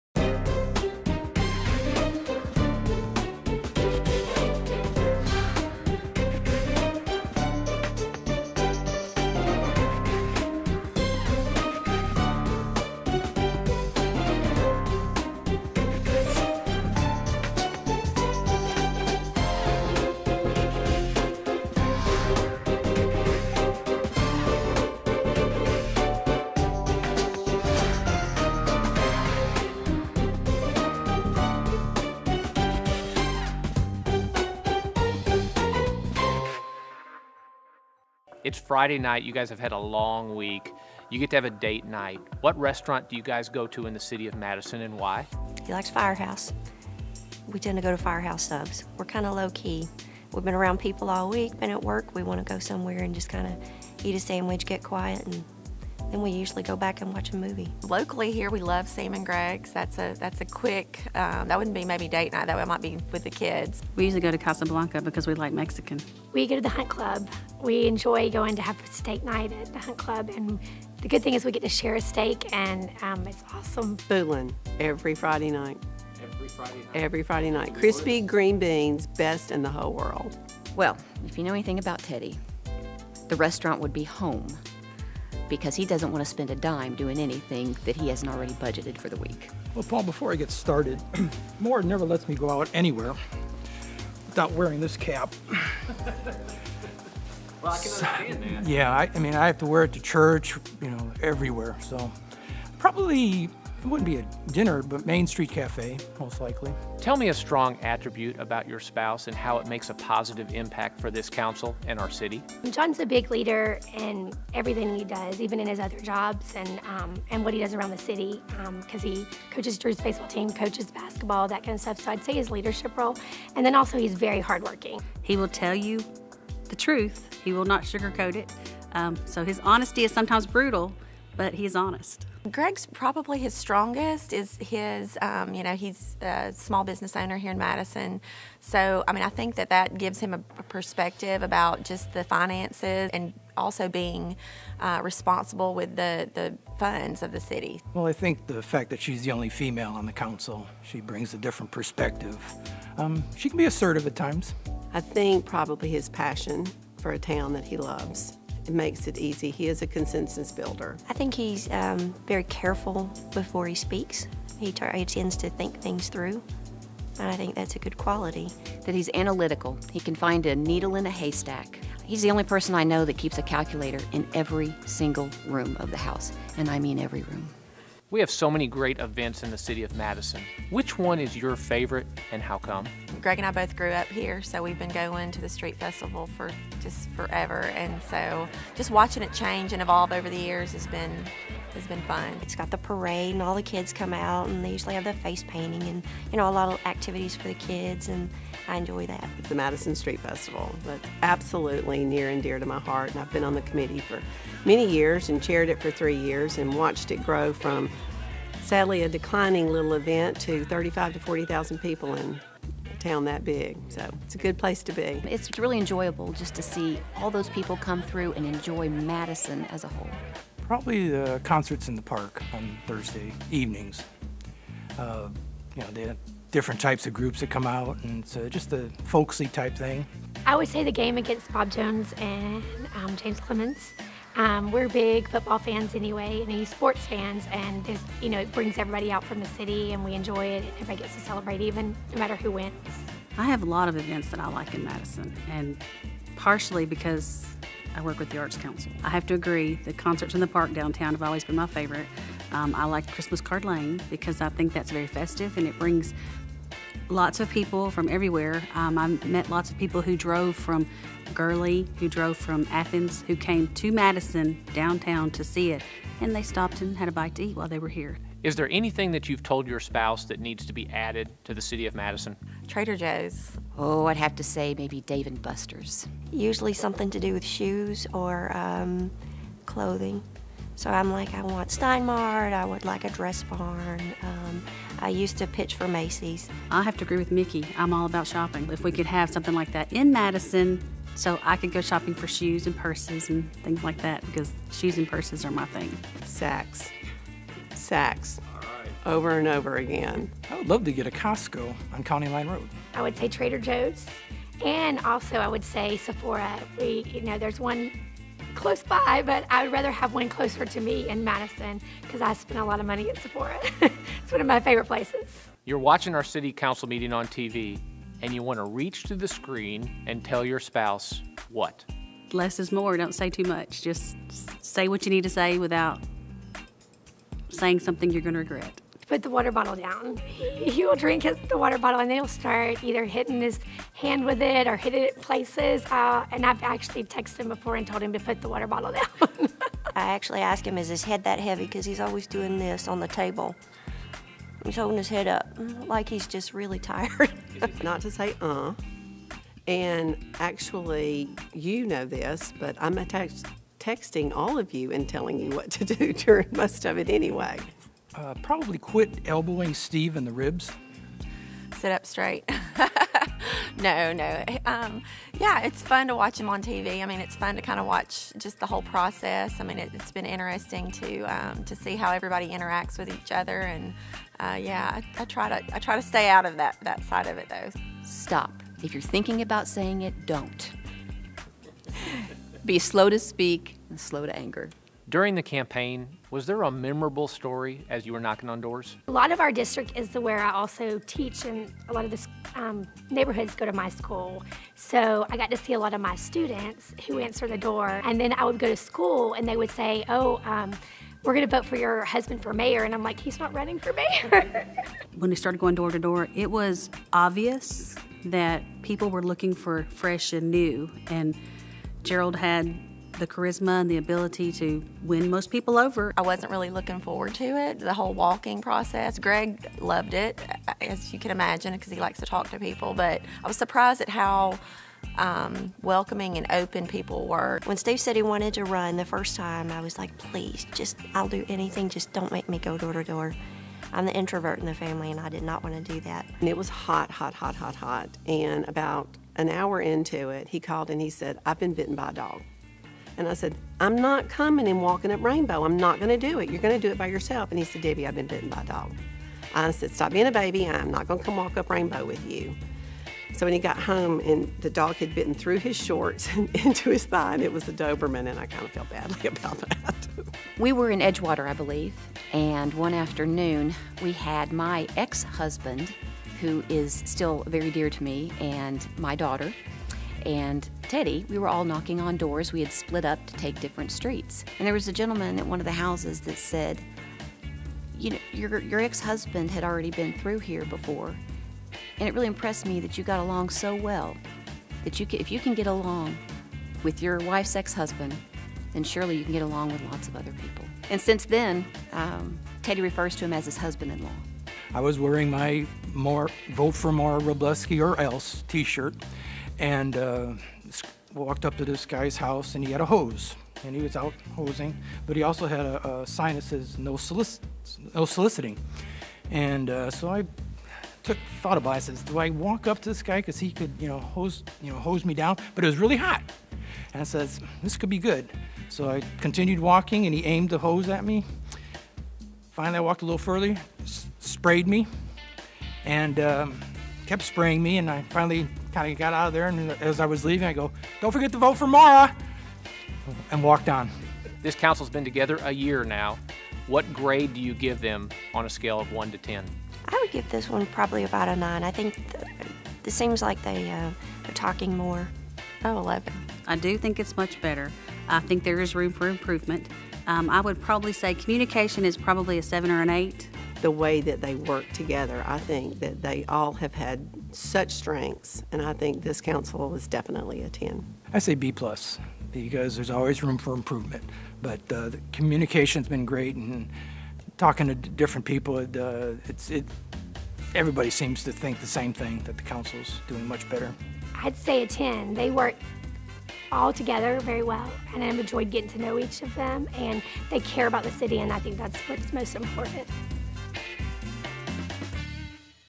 Madison Council Real Housewives and Ron 011918
Mayor Paul Finley interviews the spouses of Madison City Council members to get their views on life in politics.